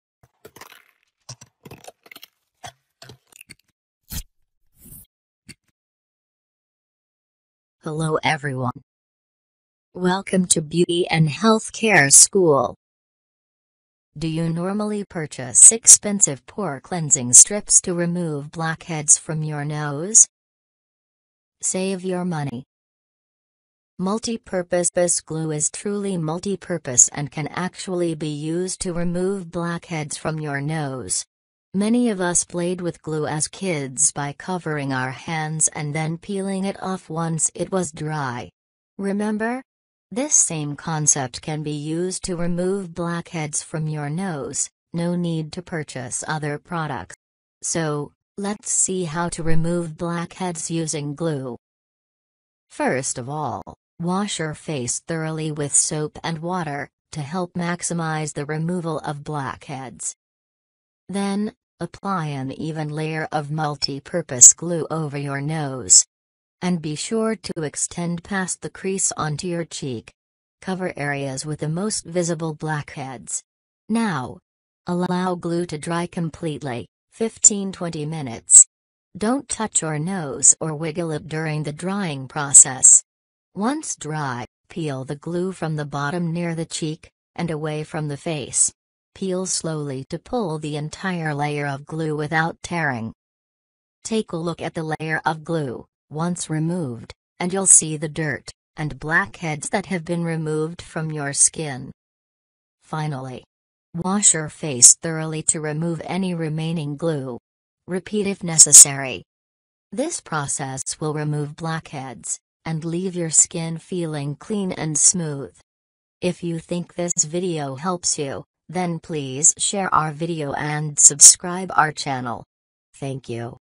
How to remove blackheads from nose with glue at home
how to remove blackheads from nose with glue

1. Wash your face thoroughly with soap and water to help maximize the removal of blackheads. Dry.

2. Apply an even layer of multi-purpose glue over your nose and be sure to extend past the crease onto your cheek. Cover areas with the most visible blackheads.

3. Allow glue to dry completely; 15-20 minutes. Don’t touch your nose or wiggle it during the drying process.

4. Once dry, peel the glue from the bottom near the cheek and away from the face. Peel slowly to pull the entire layer of glue without tearing. Take a look at the layer of glue, once removed, and you’ll see the dirt and blackheads that have been removed from your skin.

5. Wash your face thoroughly to remove any remaining glue. Repeat if necessary. This process will remove blackheads and leave your skin feeling clean and smooth!